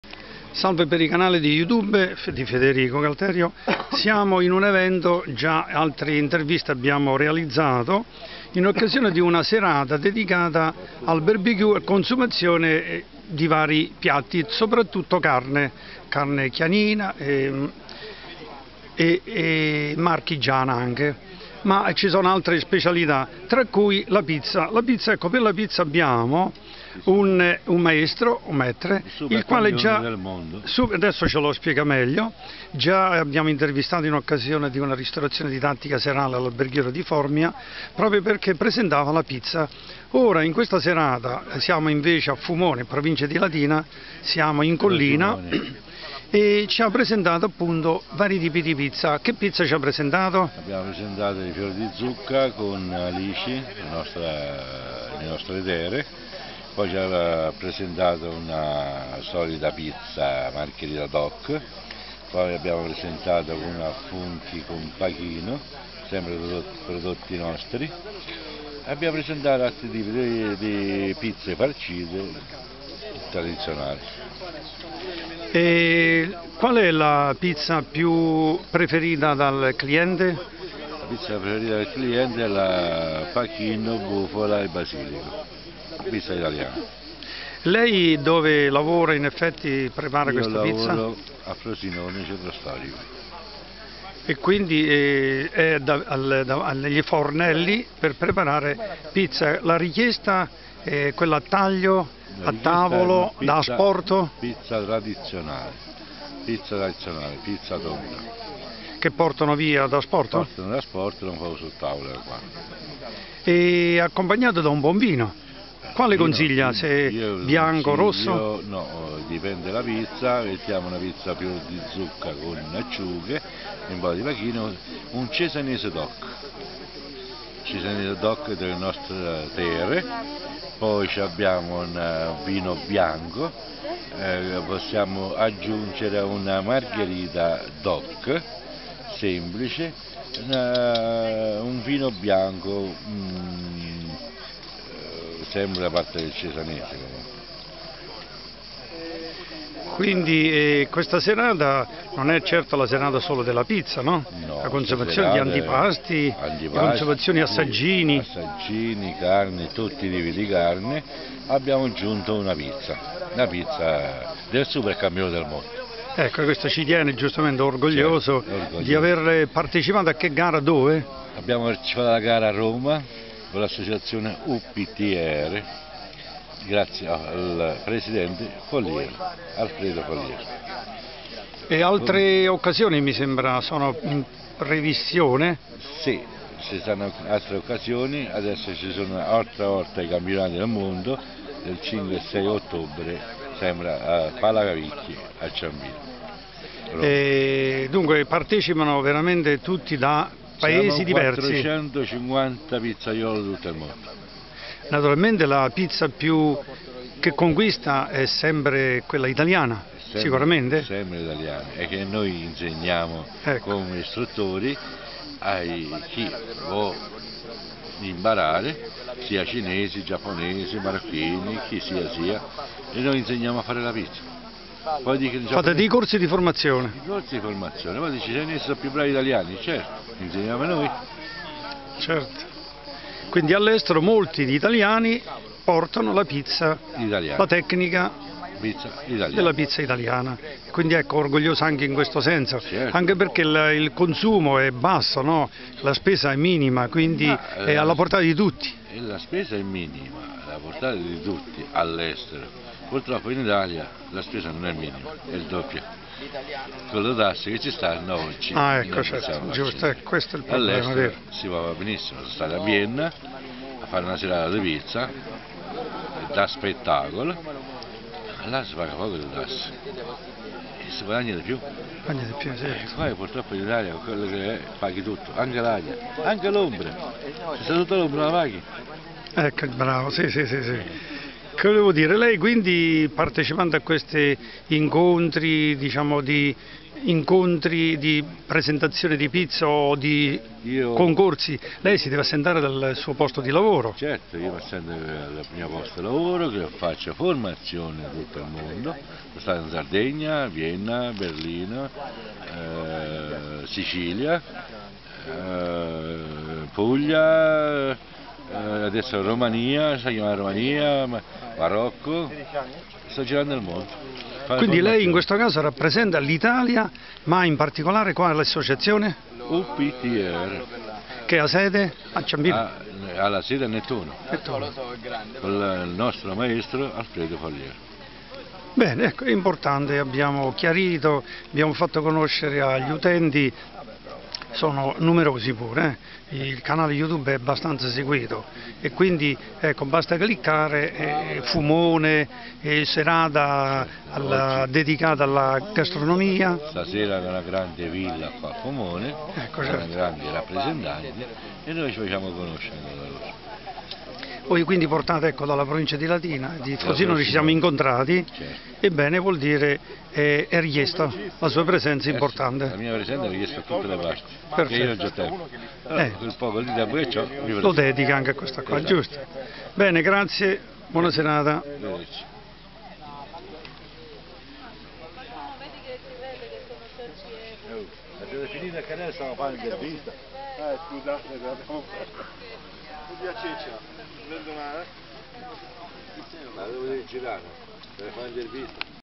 FUMONE (FR), Cena privata serale per buongustai di vini e piatti di carne
Cena enogastronomica nella semplicità di una bellezza antica e nella leggiadra armonia della natura.
Interviste agli ospiti della particolare serata.